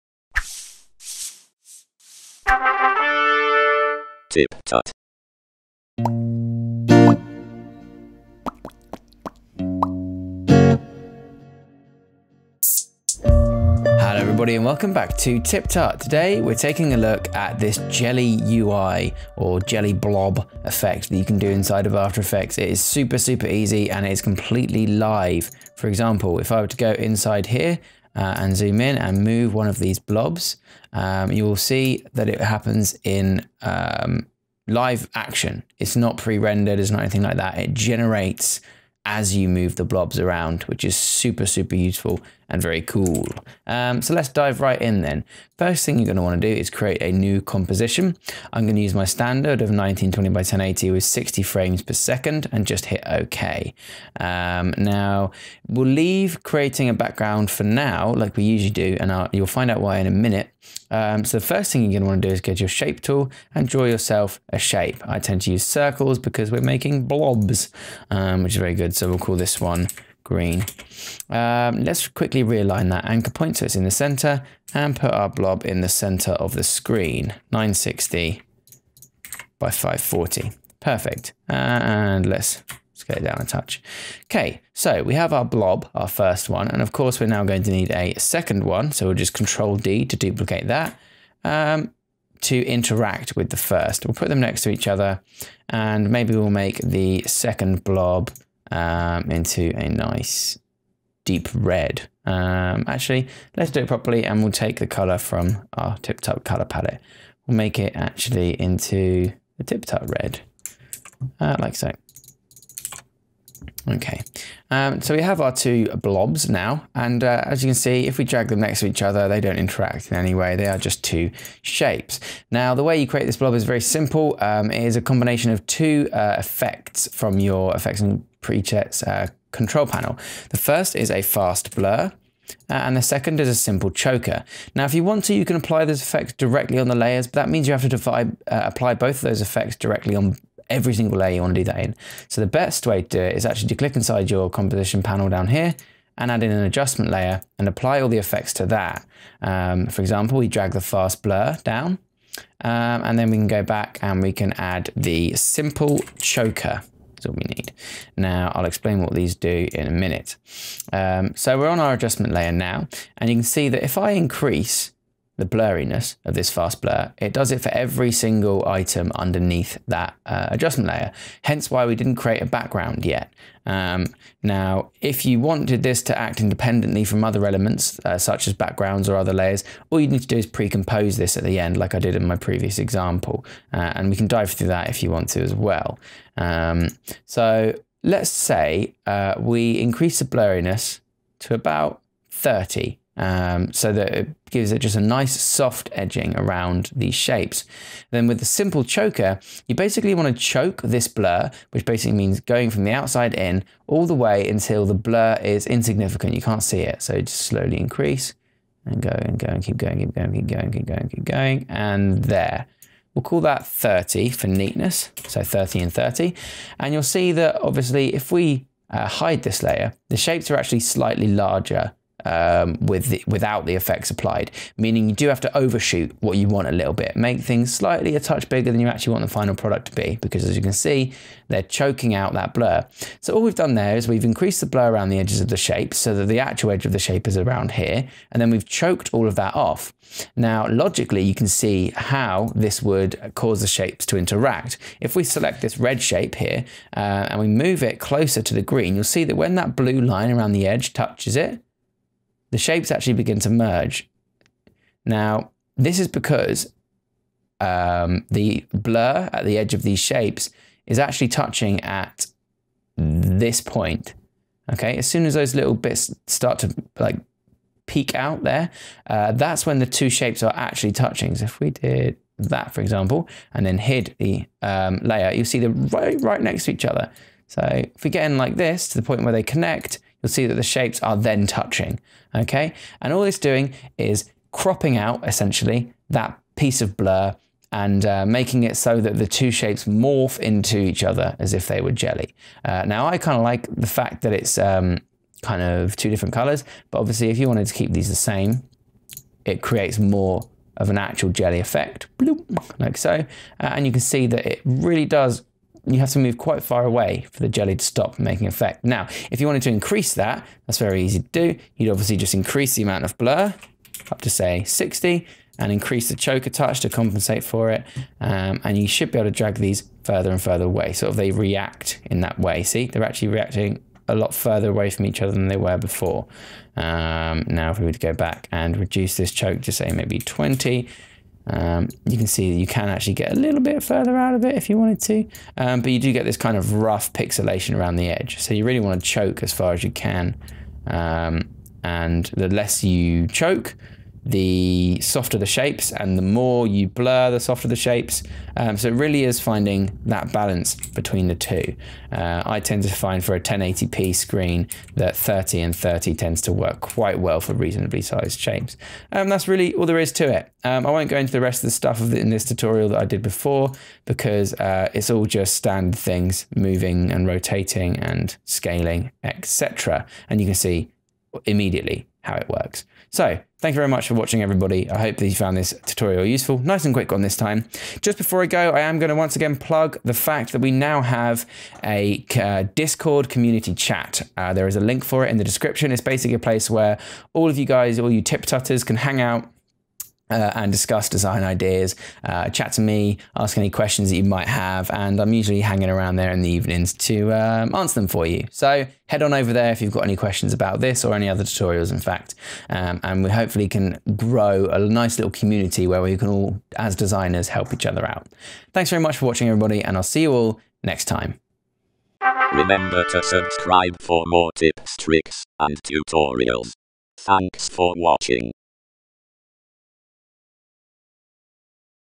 Jelly UI Effect | After Effects Tutorial
Learn how to make bloopy jelly in After Effects!